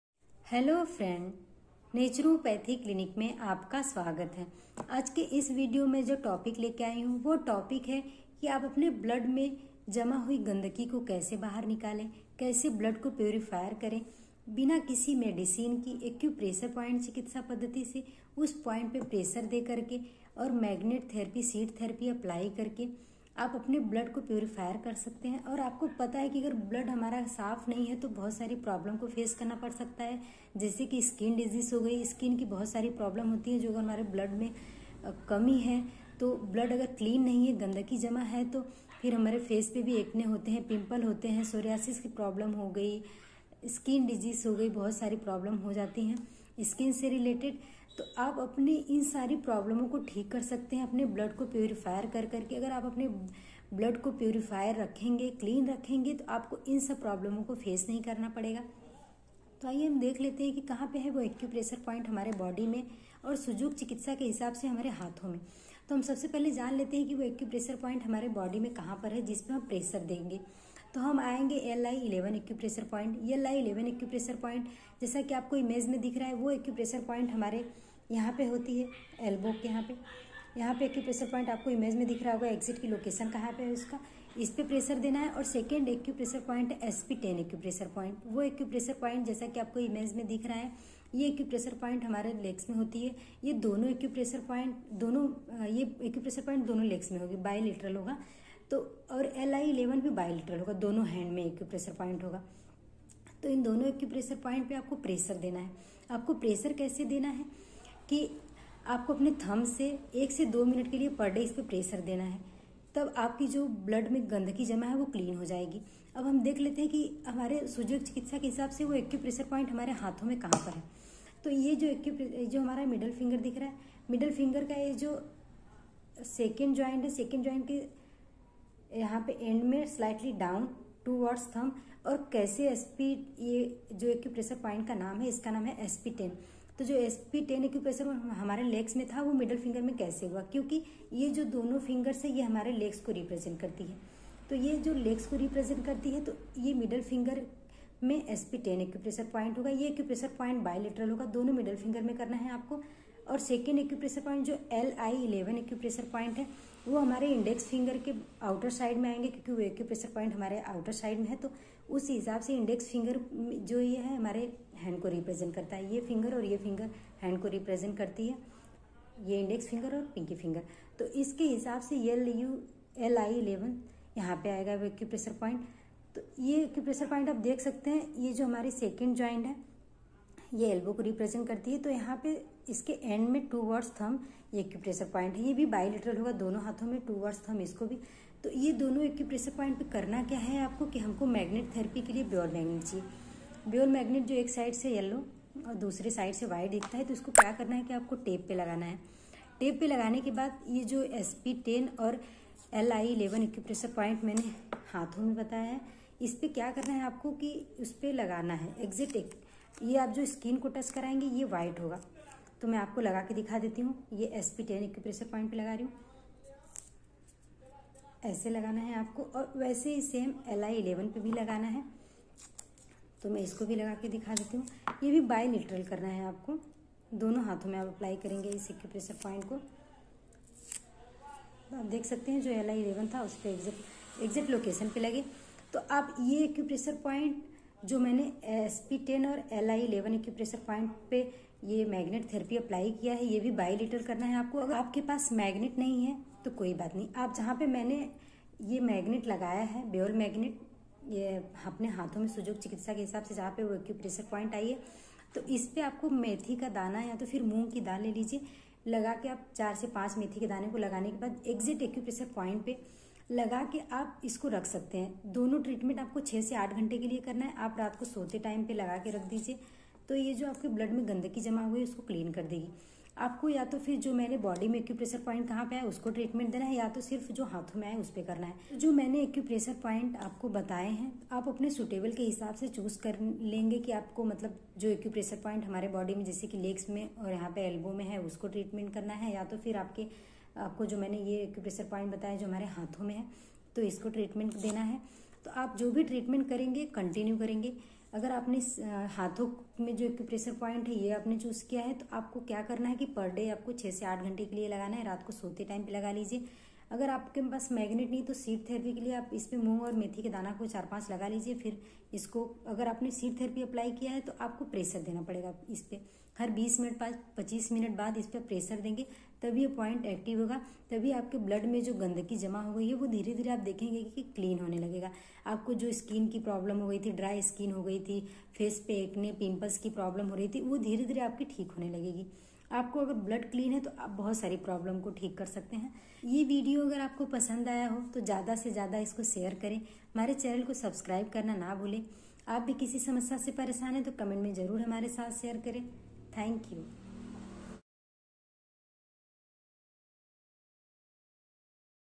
Blood Purifier treatment by Acupressure points.खून की गंदगी को कैसे करें साफ।
ब्लड में गंदगी से फोड़े-फुंसी, पिंपल और चर्म रोग हो जाते हैं। जल्दी थक जाना, वजन कम हो जाना, पेट की दिक्कतें आदि भी ब्लड में गंदगी की वजह से होती हैं।